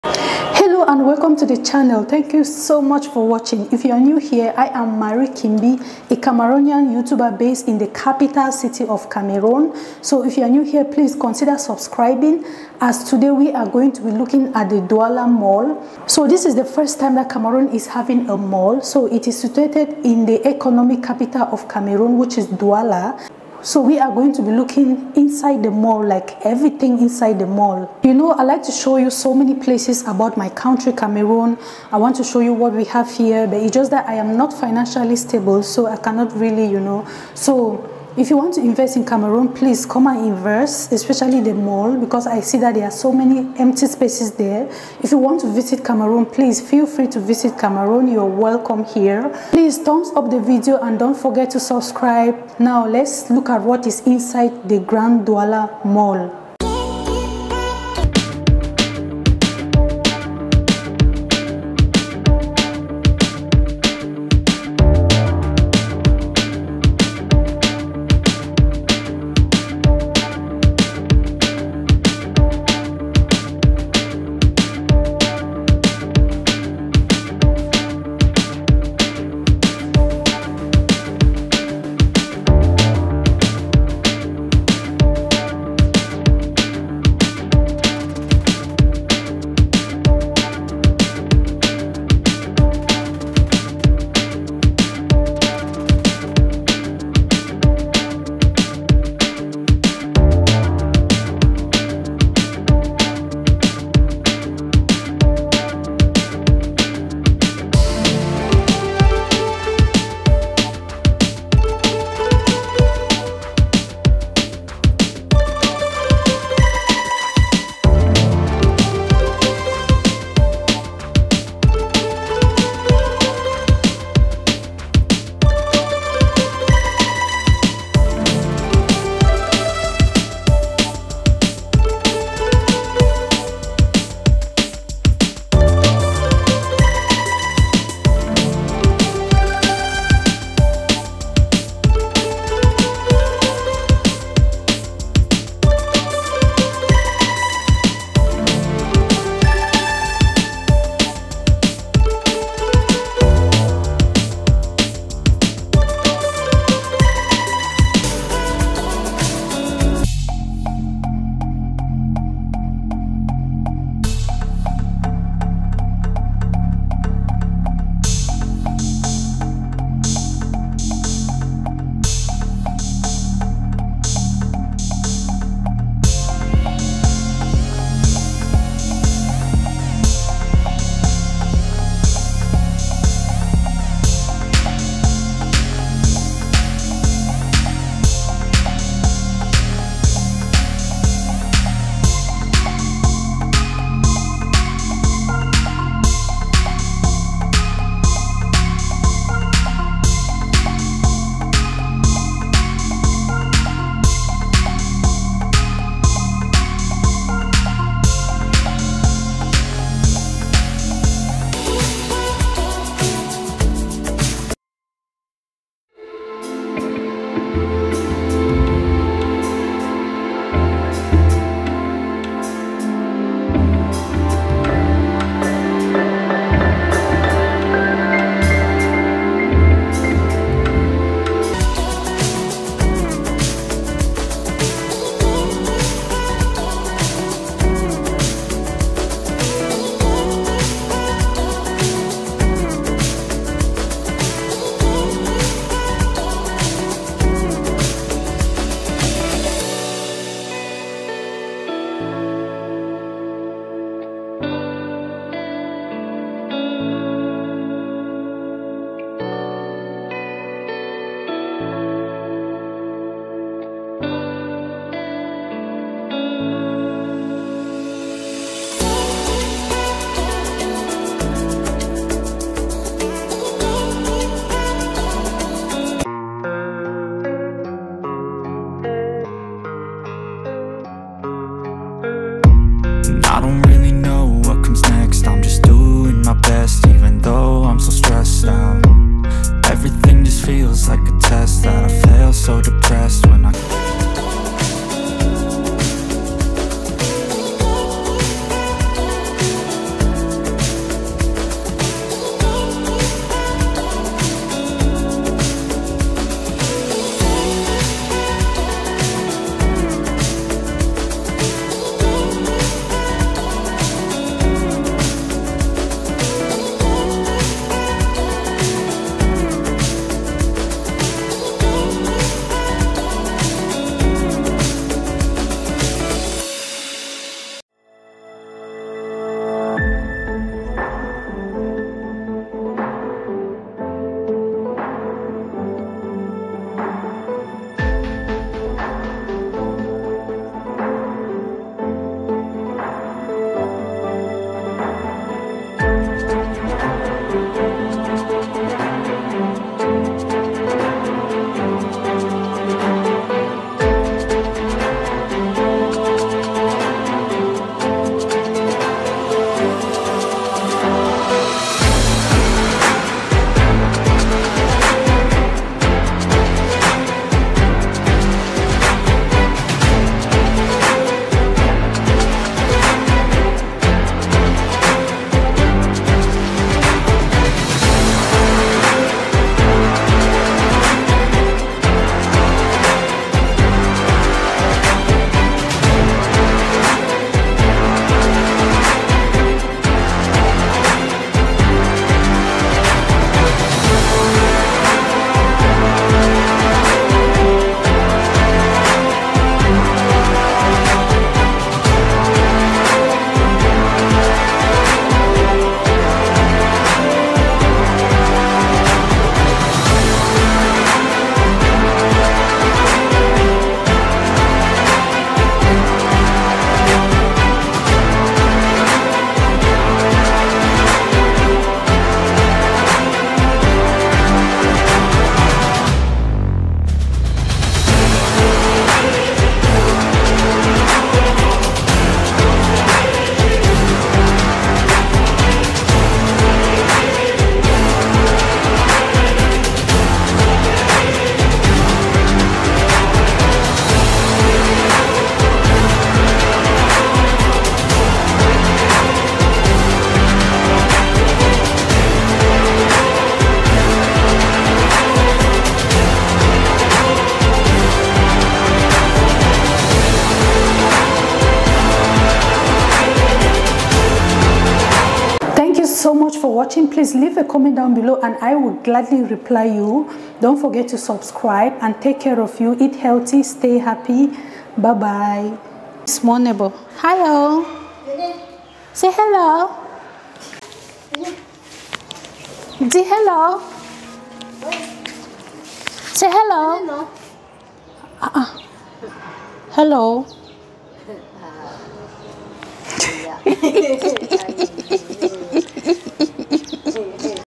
Cameroon build one of the biggest mall in central Africa | inside the Douala Grand Mall
◇Please keep me in your prayers 🙏 😢